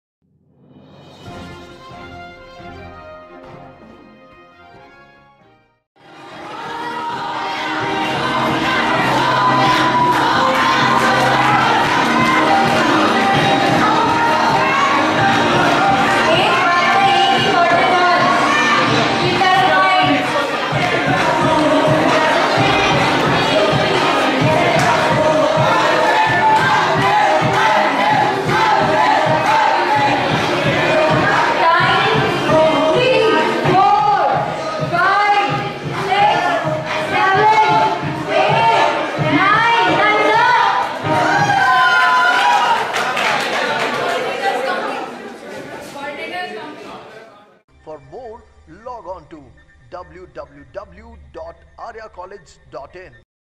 Zephyr 2017: (Arya 1st Old Campus)
Zephyr is the Annual Tech Fest of ECE & EE Department of AryaCollegeOfEngineeringandIT. The Honorable Vice Chancellor of Rajasthan Technical University, Prof. N.P. Kaushik visited the AryaCollegeJaipur. The activities which were part of this event were Robo Hurdle Race, Robo Relay Race, Robo Soccer, Robo War, Musimania, and Poster Making. These all activities were conducted on 9th August 2017 at Arya1stOldCampus. This event gave students a platform to showcase there TechnicalSkills and polish them with more Enthusiasm.